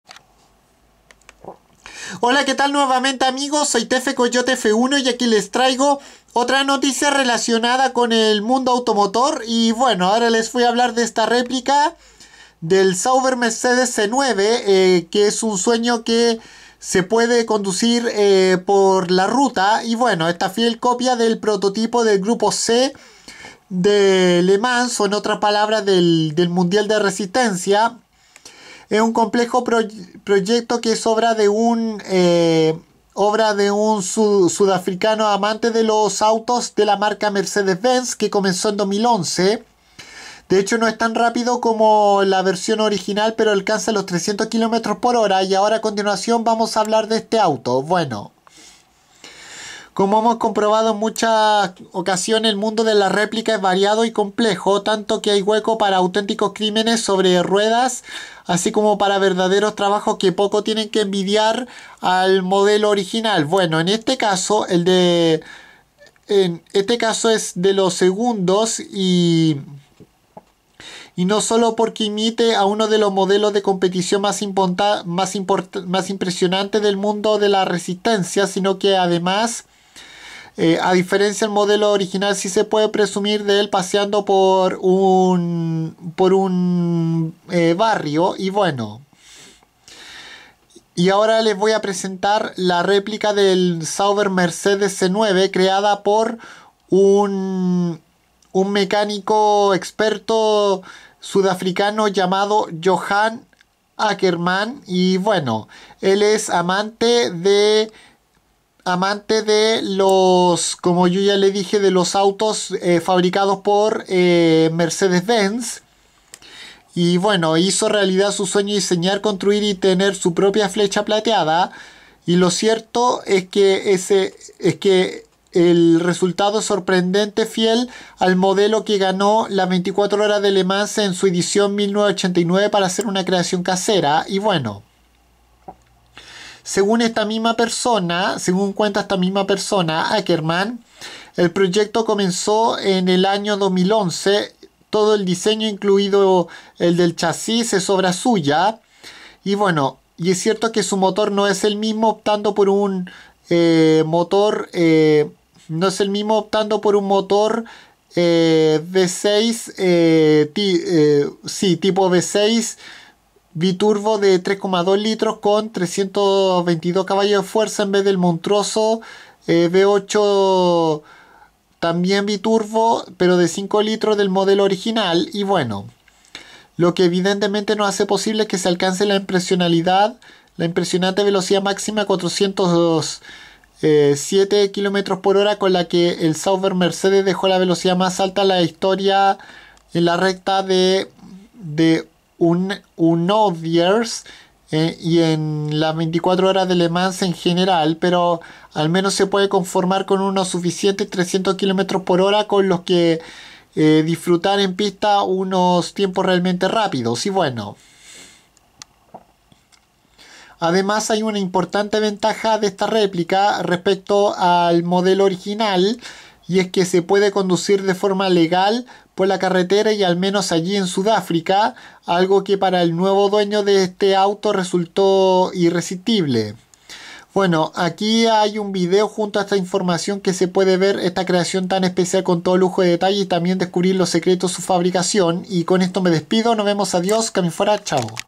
Réplica de un Sauber Mercedes C9 creada en Sudáfrica
En este vídeo les cuento como un mecánico experto fanático de la marca alemana Mercedes-Benz construyó su propio Sauber Mercedes C9 desde 2011 y que ademas es una fiel copia del modelo que ganó en 1989 las 24 Horas de Le Mans